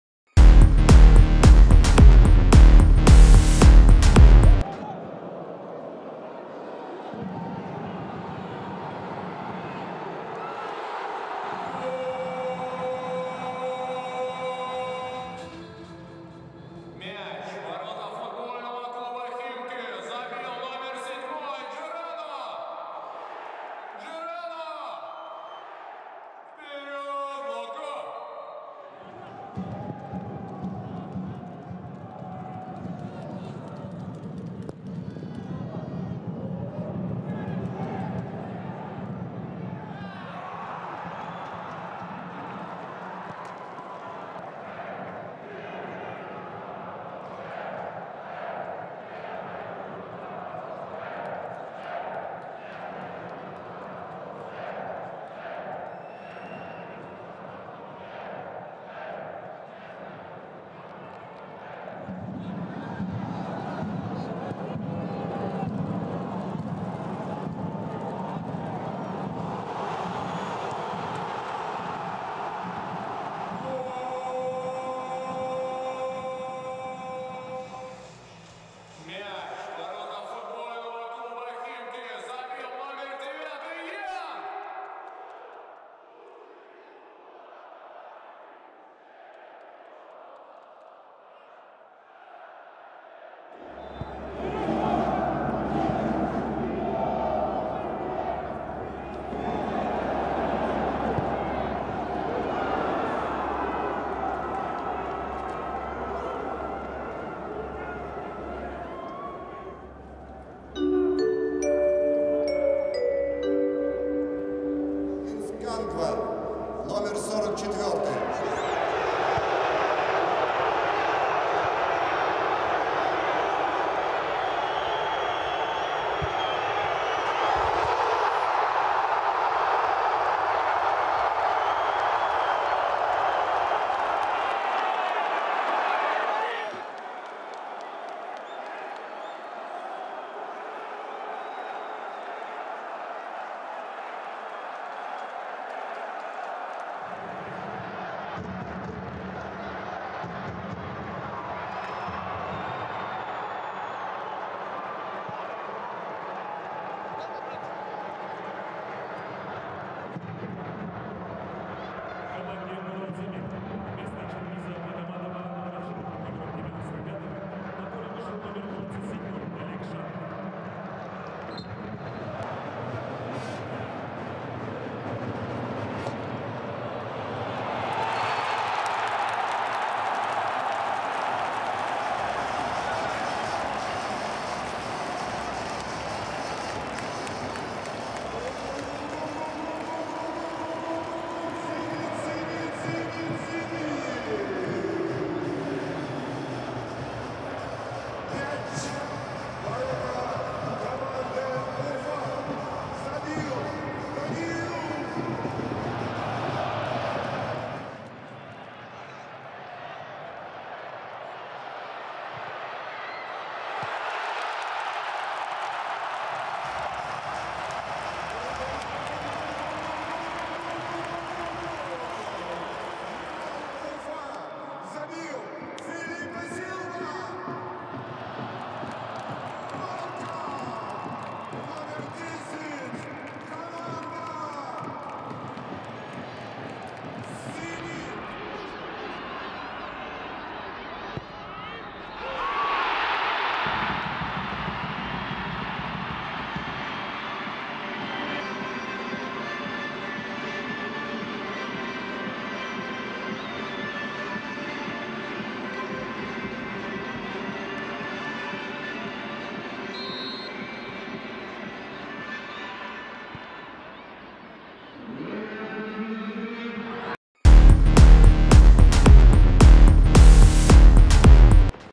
Top Goals, Week 20 | RPL 2021/22
Watch best goals of Week 20:

0:00 Start
0:07 Gyrano Kerk Week 20, Lokomotiv vs FC Khimki, 8’
0:32 Didier Lamkel Ze Week 20, Lokomotiv vs FC Khimki, 72’
1:05 Jan Kuchta Week 20, Lokomotiv vs FC Khimki, 80’
1:37 Kings Kangwa Week 20, Krylia Sovetov vs Arsenal, 13’
2:01 Quincy Promes Week 20, Dynamo vs Spartak, 37’
2:30 Eric Bicfalvi Week 20, FC Krasnodar vs FC Ural, 79’
2:55 Danil Krugovoy Week 20, Zenit vs FC Ufa, 50’
3:30 Malcom Week 20, Zenit vs FC Ufa, 67’
4:01 Daniil Utkin Week 20, Akhmat vs Rubin, 3’